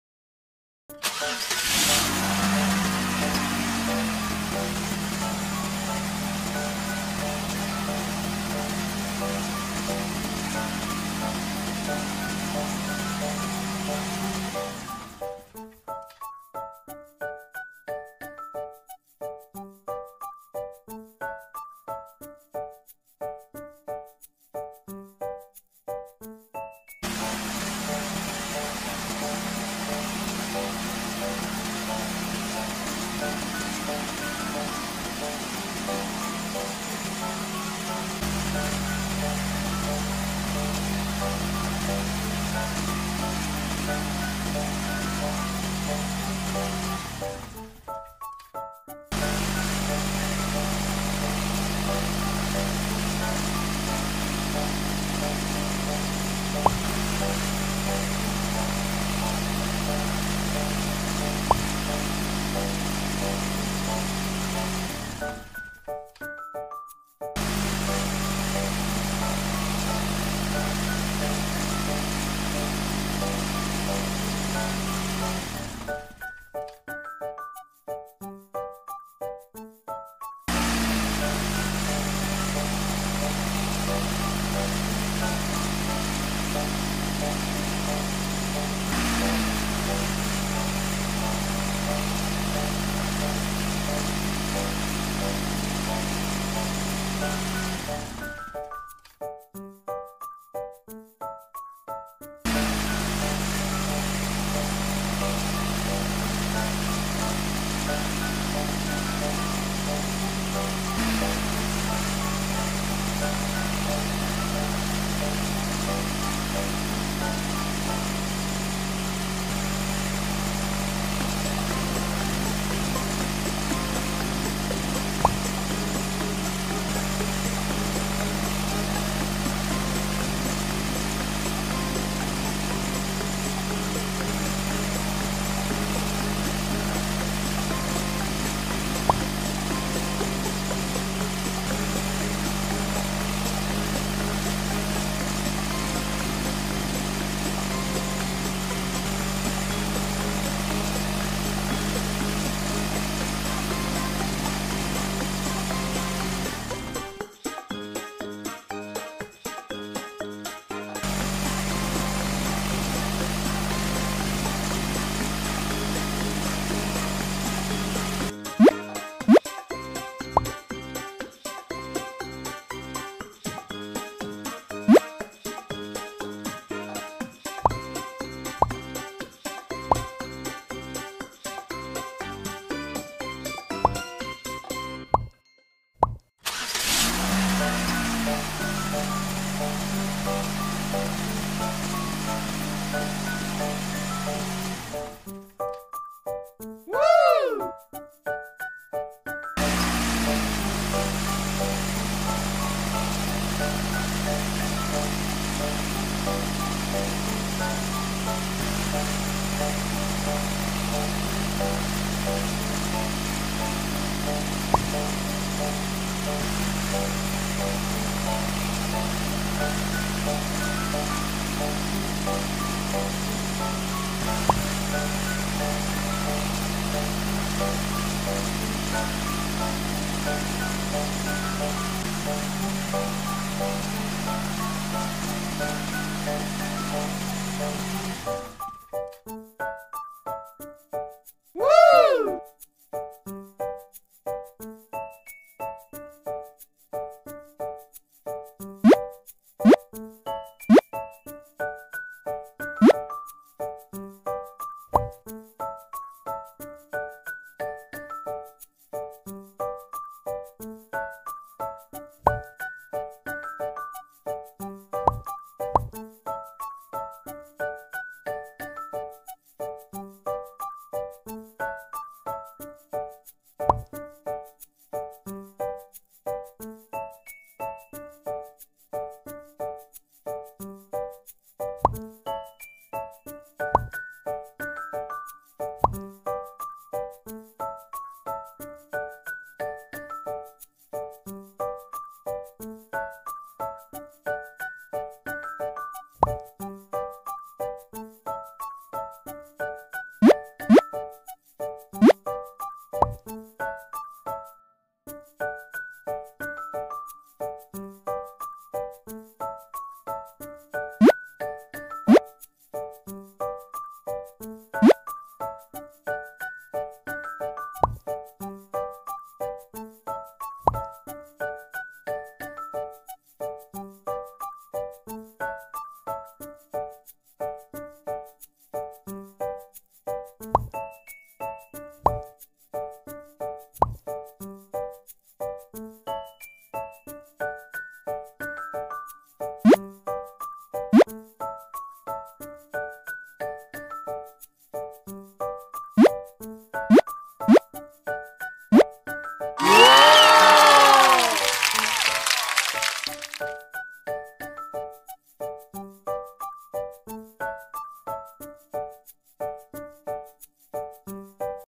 Building Castle, Giant Castle, Truck, Trailer, Construction Workers, Construction Engineers
Let's accompany the Engineers and Construction Workers and watch them complete the castle!!!